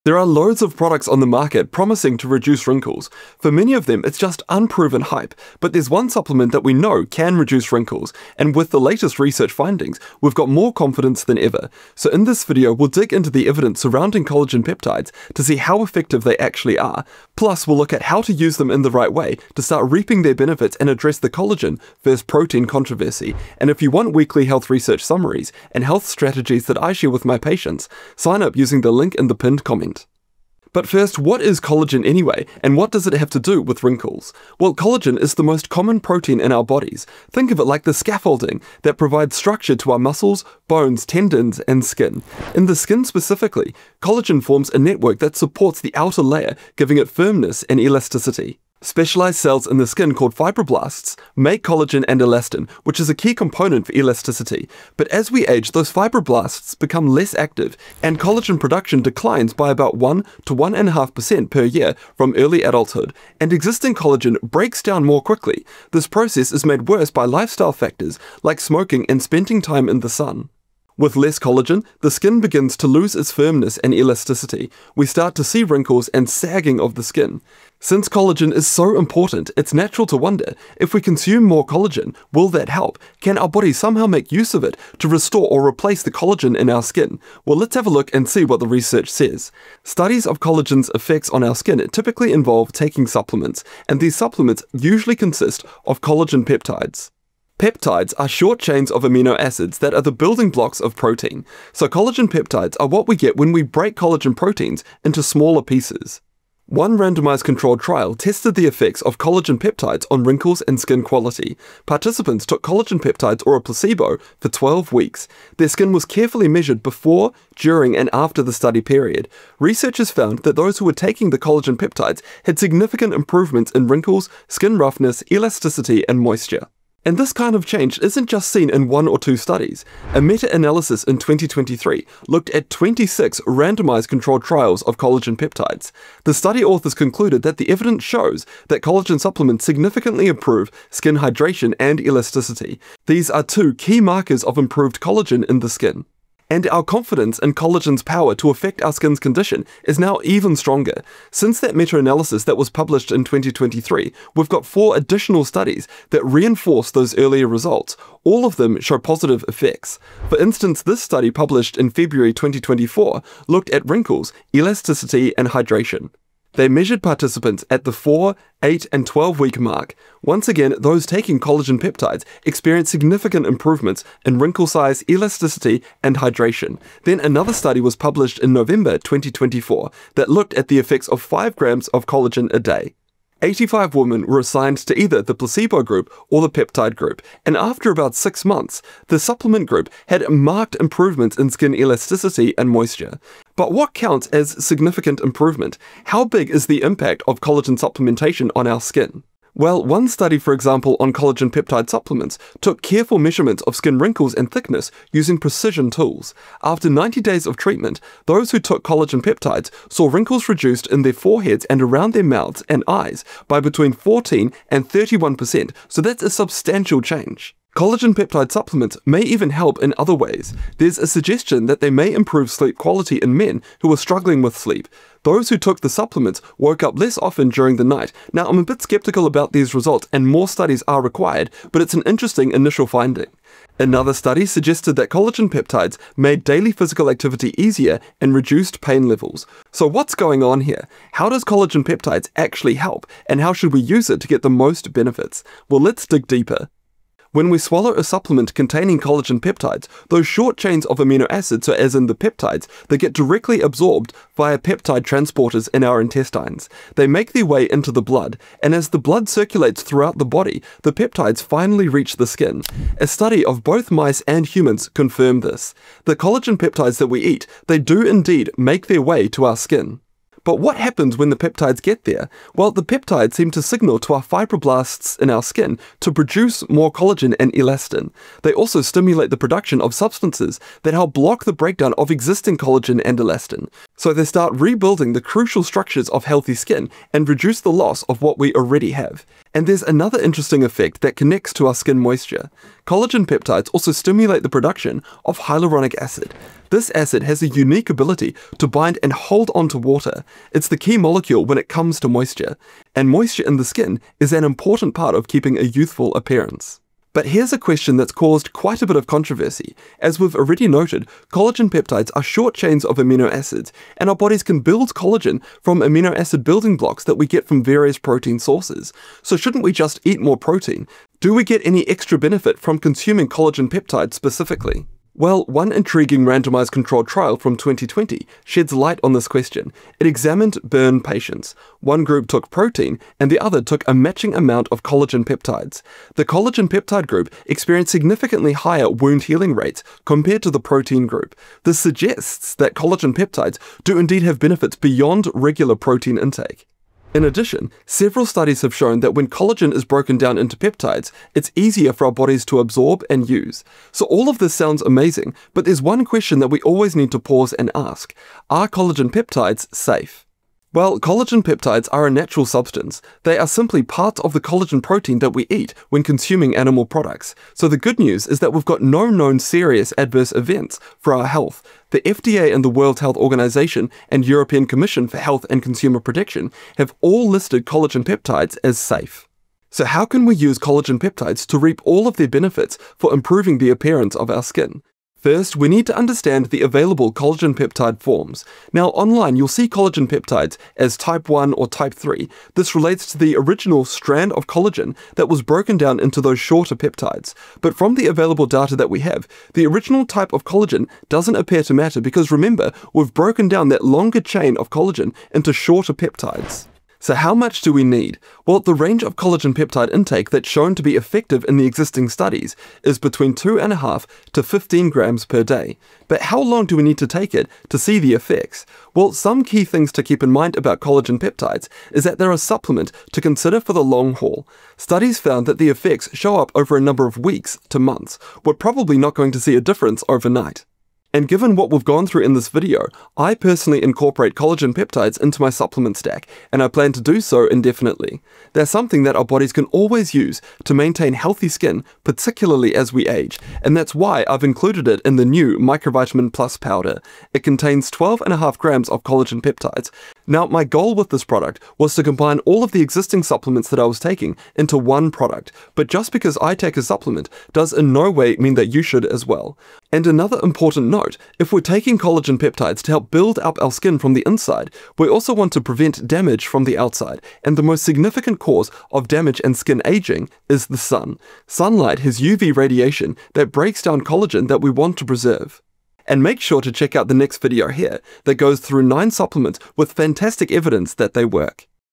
Can Collagen Peptides Reverse Skin Wrinkles? New Studies
For weekly health research summaries and extra insights, sign up

Thumbnail by James Kelly
Video edited by Troy Young
Script by John Milliken